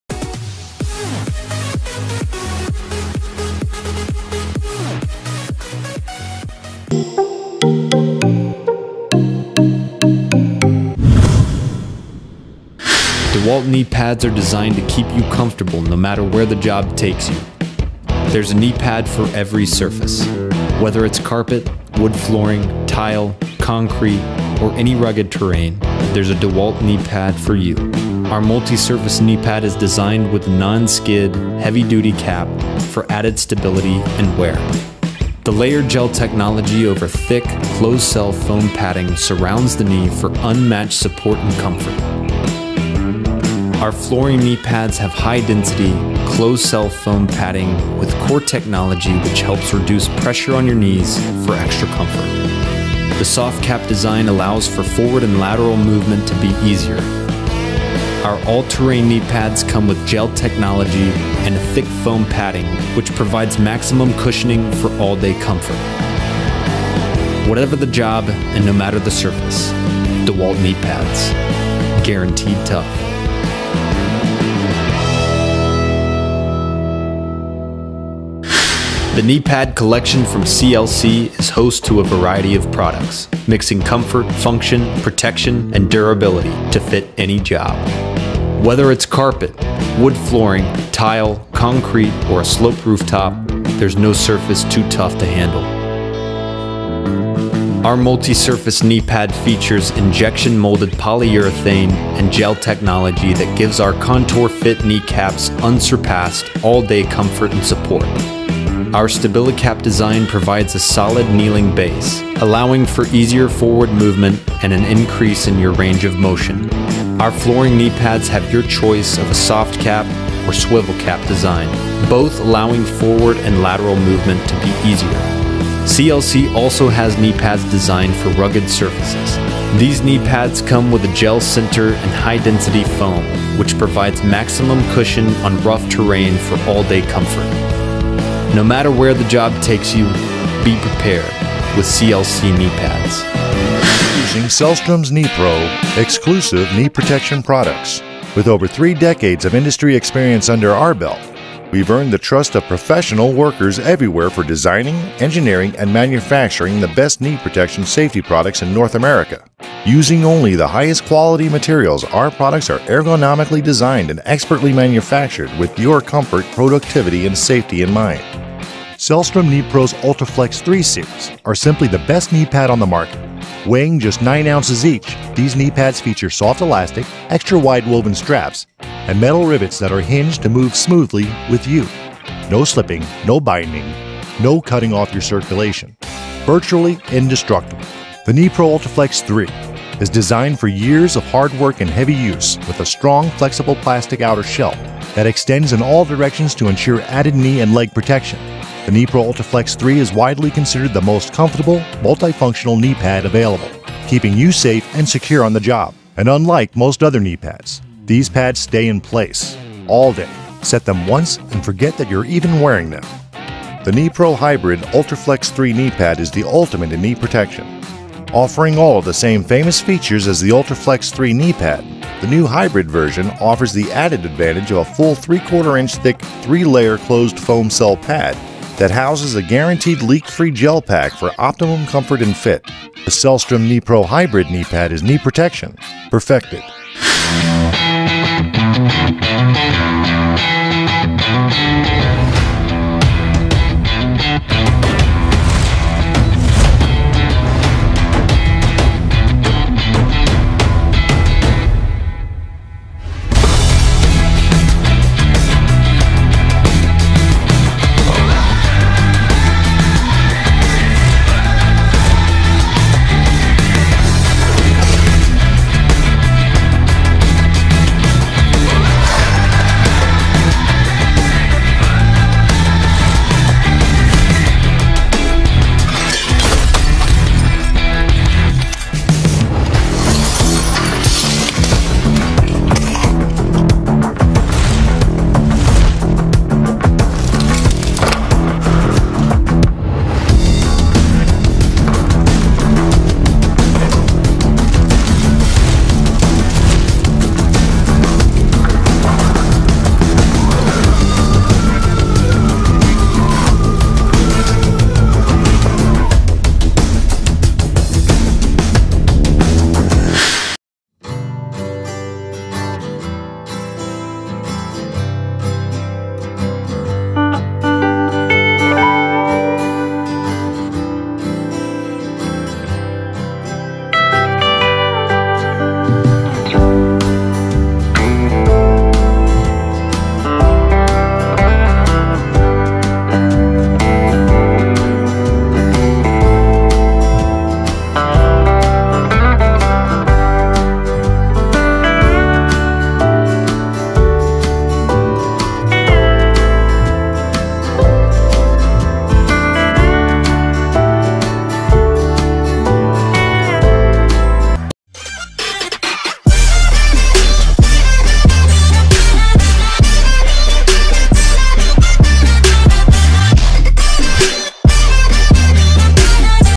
Top 5: Best Knee Pads in 2024
5. DEWALT DG5204 Professional KneepadsDEWALT DG5204 Professional Kneepads

4. Sellstrom S96110 Knee Pro Ultra Flex III Series

3. NoCry Professional Knee Pads

2. Gift Comfortable Knee Pads

1. VUINO Heavy Duty Knee Pads

Are you searching for the best knee pads? A knee pad can be incredibly useful to control pain in the knees. The best knee pads are elusive and can be befuddling to make sense of.

We see how significant you love laying tile! To make it less hopeless, ensure you're wearing the best knee pads you can manage.

      leave us a message (takebest01 at gmail dot com) so we can